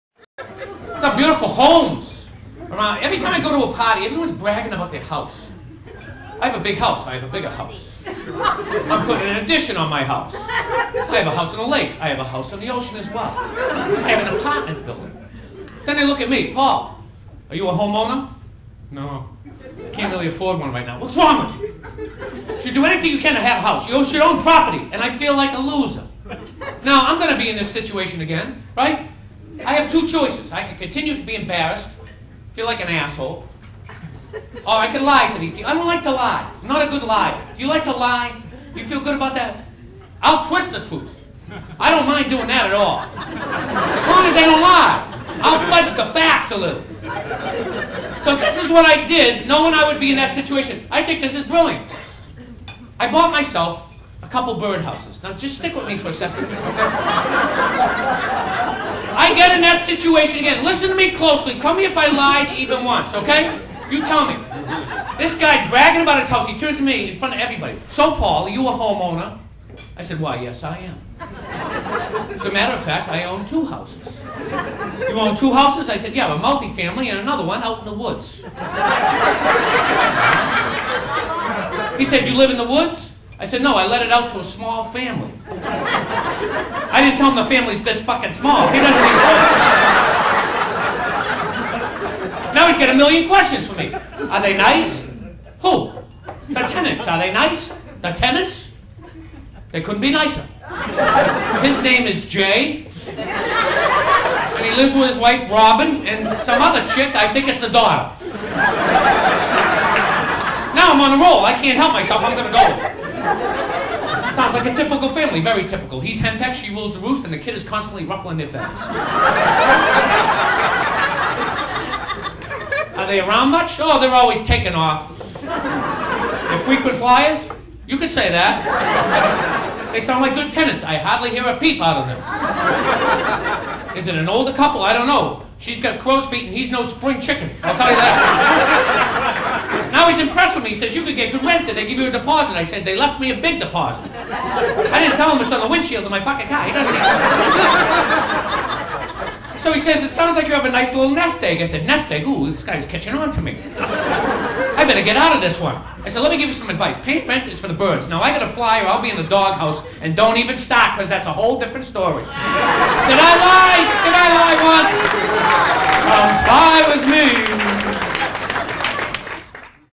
Birdhouses
Comedian Paul D'Angelo recorded live at the Hermosa Beach Comedy & Magic Club, Hermosa Beach, CA on April 21, 2005